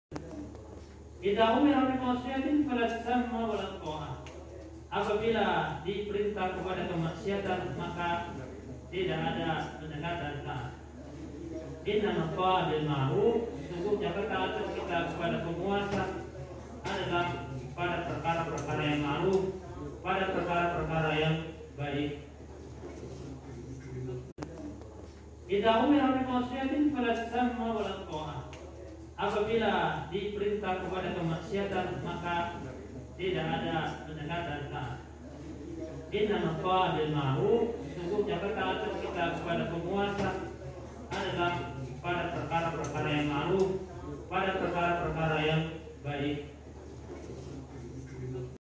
orang utan ikut sholat jum'at
di kalimantan memang memang terkenal banyak berbagai jenis hewannya.termasuk hewan yang satu ini ya itu orang utan,hewan yang satu ini termasuk hewan yang paling di lindungi.hari ini tgl 24 /12/2022 ada seekor orang utan besar masuk di kerumunan jamaah yang sedang melaksanakan sholat jum'at,kejadian ini di areal tambang batu bara perusahaan PT,Pama.kaltim.